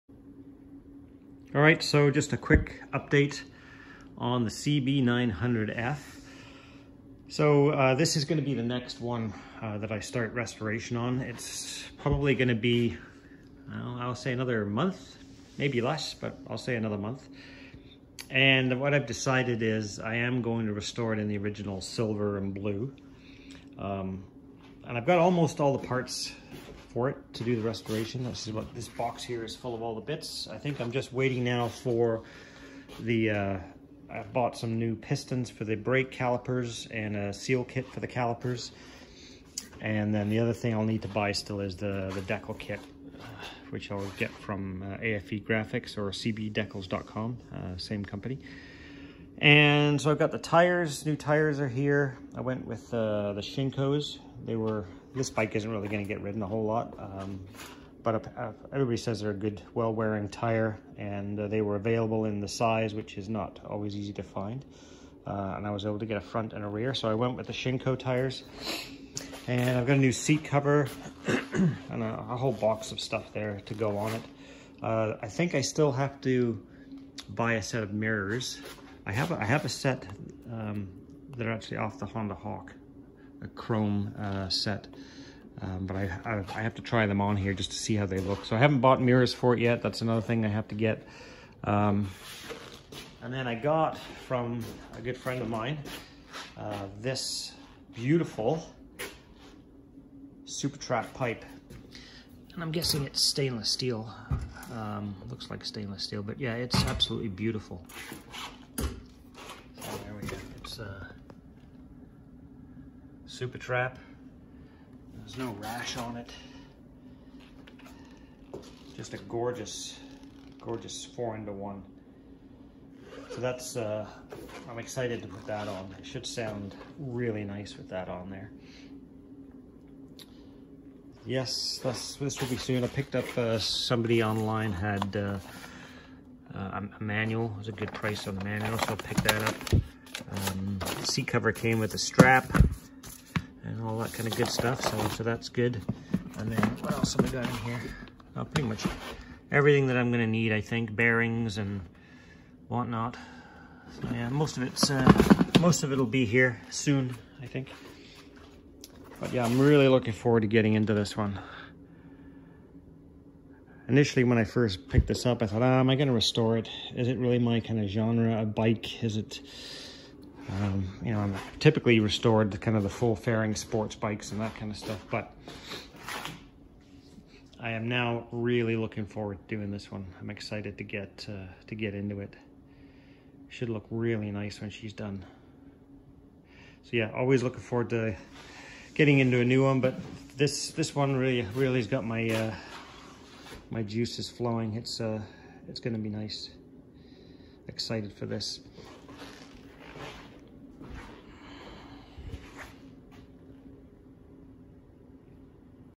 1982 Honda CB900F Super Sport - UPDATE on Restoration
Here is an update on the pending restoration of the CB900F Super sport